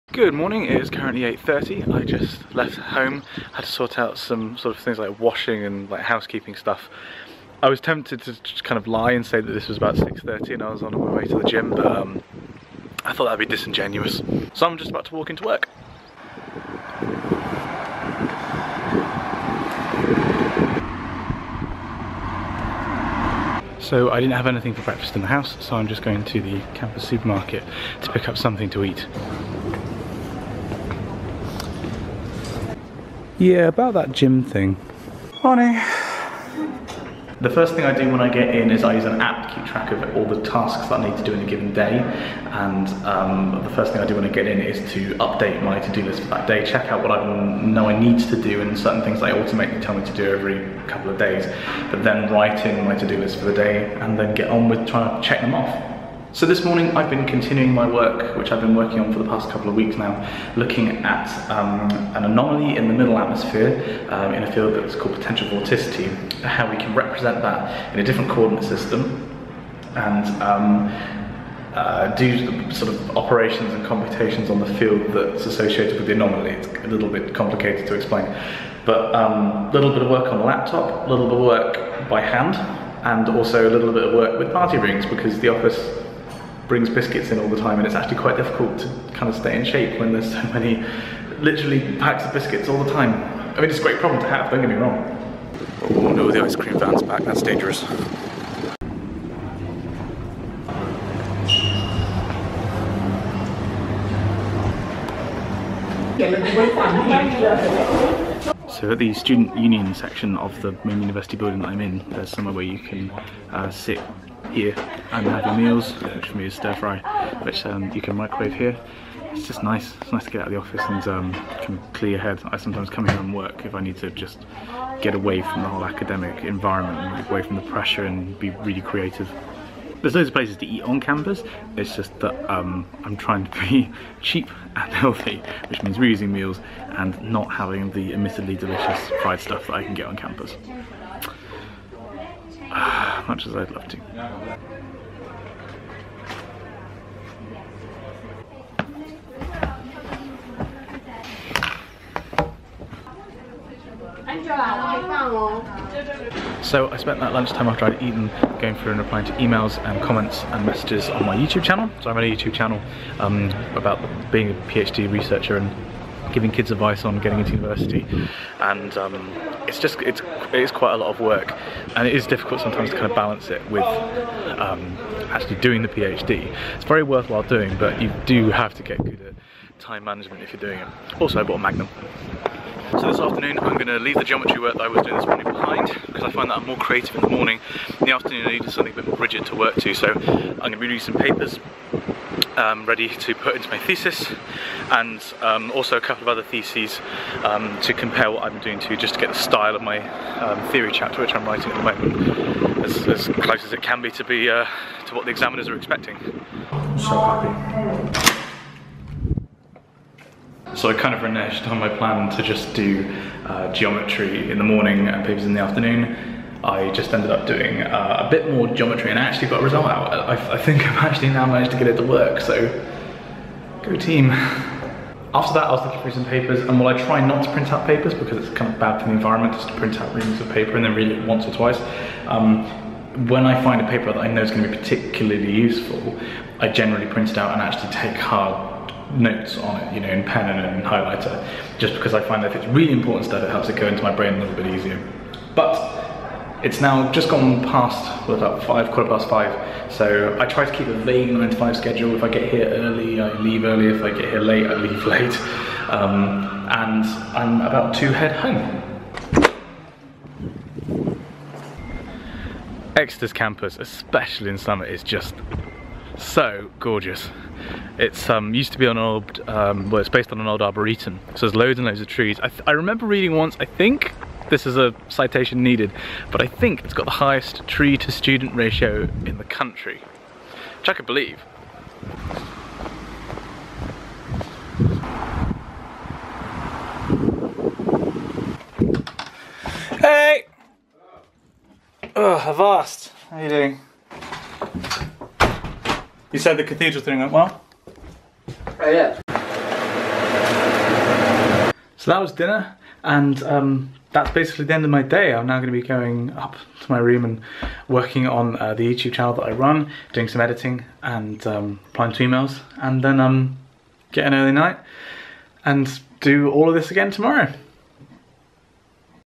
A Day in the Life: Simon Clark - PhD Study at the University of Exeter
Ever fancied yourself as a PhD student? We caught up with the Mathematics department’s Simon Clark to find out what makes an average day in the life for him as a PhD candidate here at Exeter.